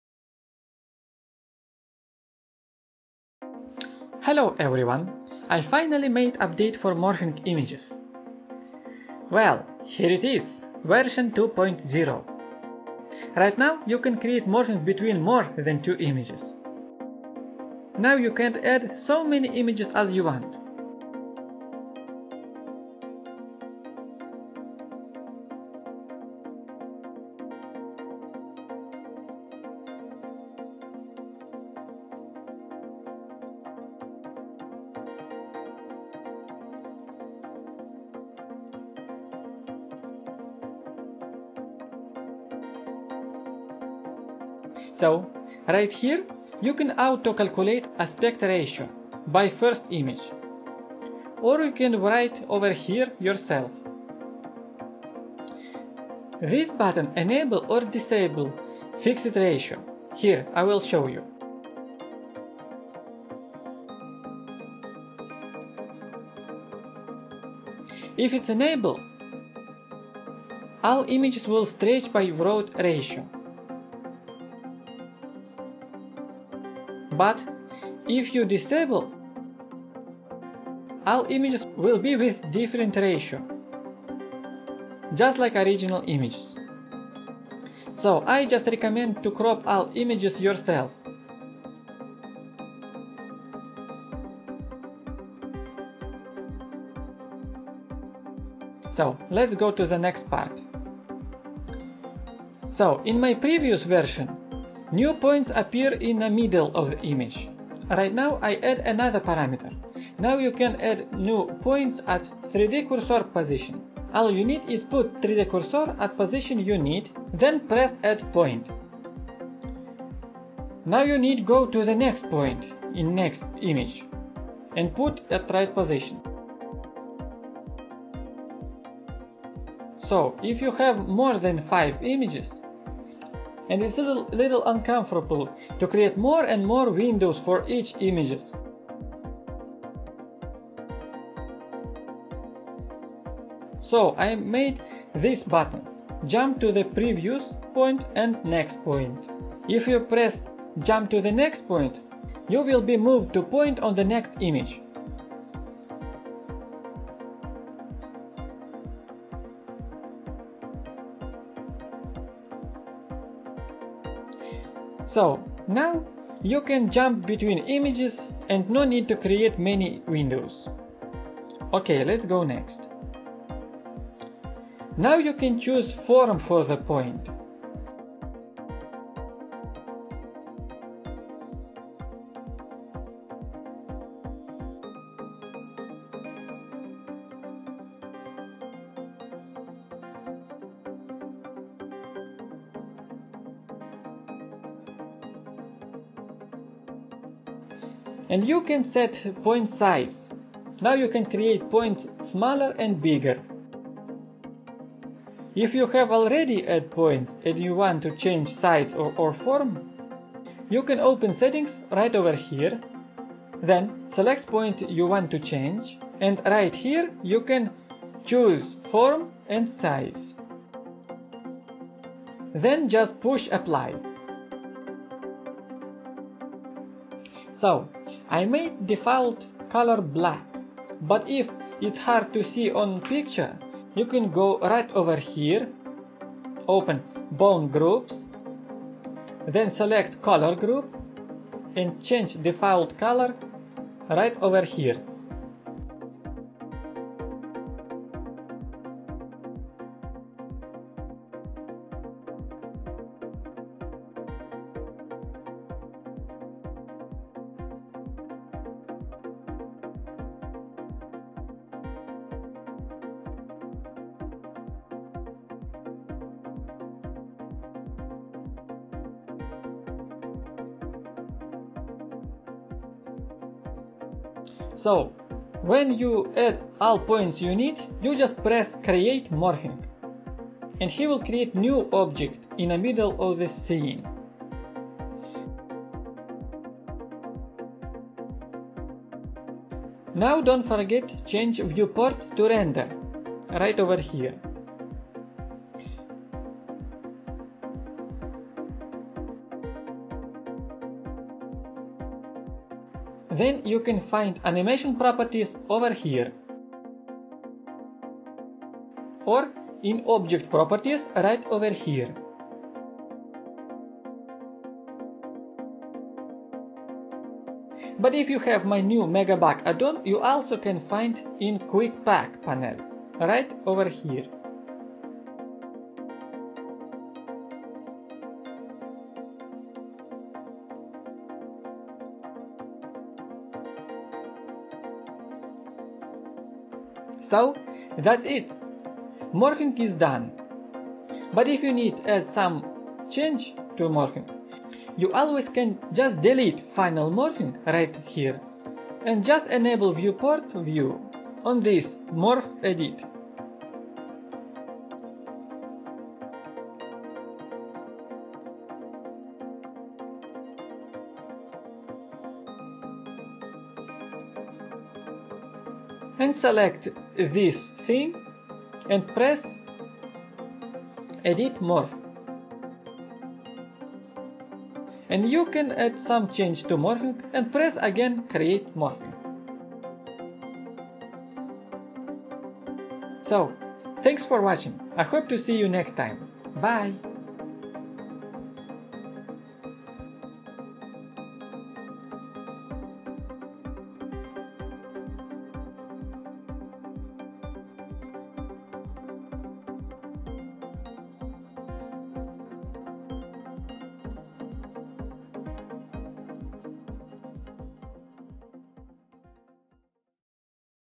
Morphing Images v2.0 | Add-on for Blender 3.0, 3.3 | 3DMish
Hello everyone!!!
I finally updated my morphing add-on for Blender.
I tested it on Blender 3.0 and 3.3.